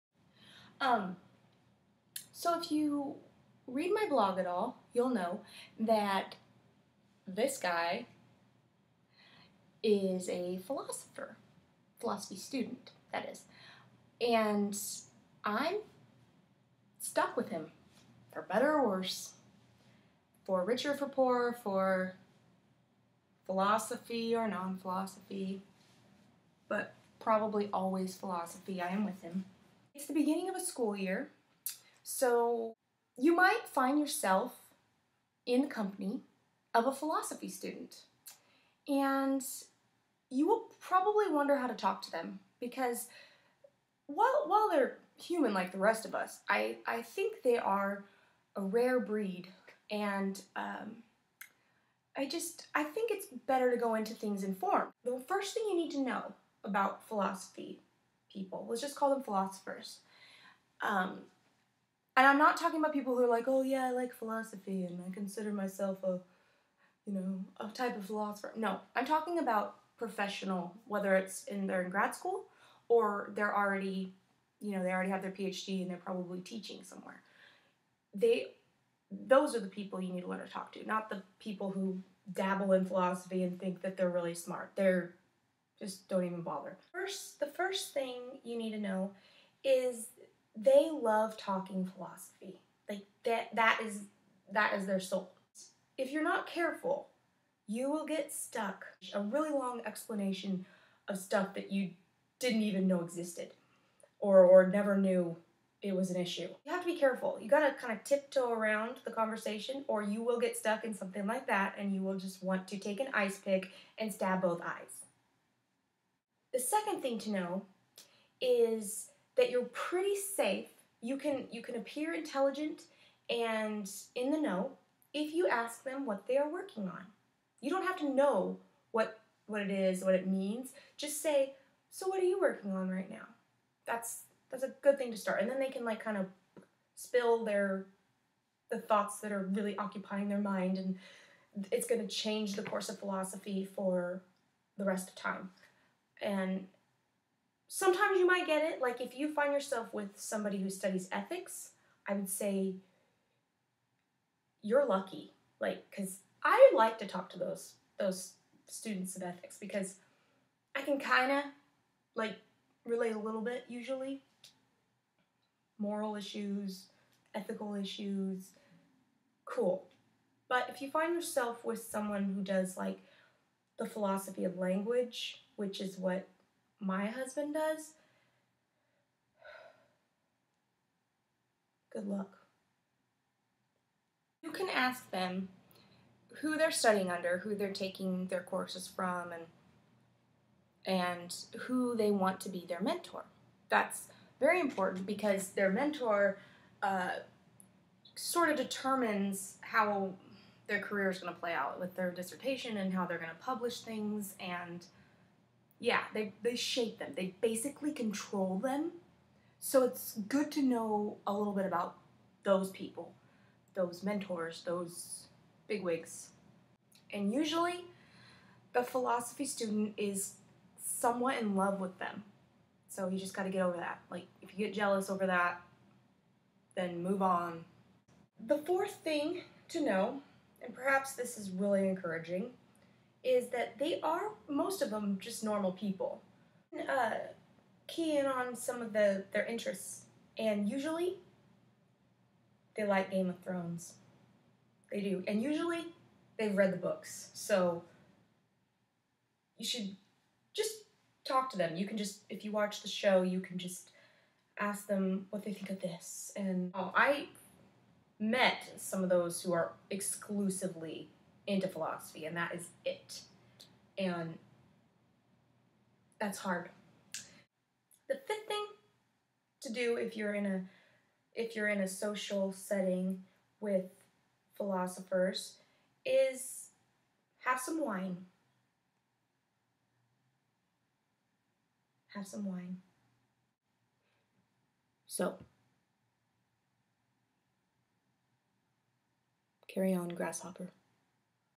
How To Talk To Philosophers
I will coach you on the delicate art of communicating with a philosopher/philosophy student in any social setting.

Remember, philosophers are tricky creatures and you should always proceed with caution. I know, because I'm married to one.